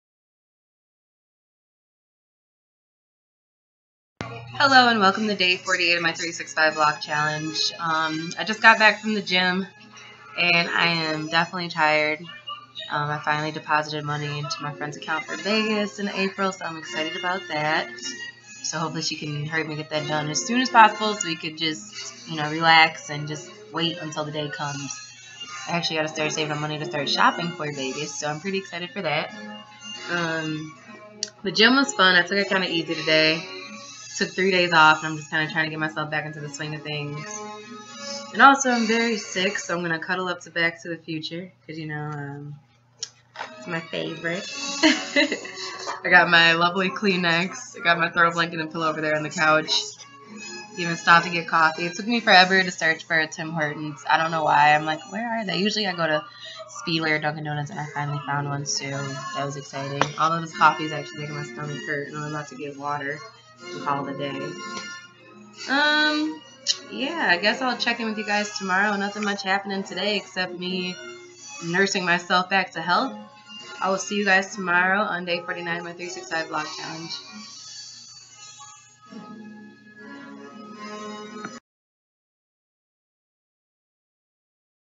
Kay Mo's 365 VLOG Challenge - Gym, BTTF, and Coffee Talk!
Day 48! For the most part, just having a relaxing day. The gym went smoothly now watching BTTF 1,2, & 3!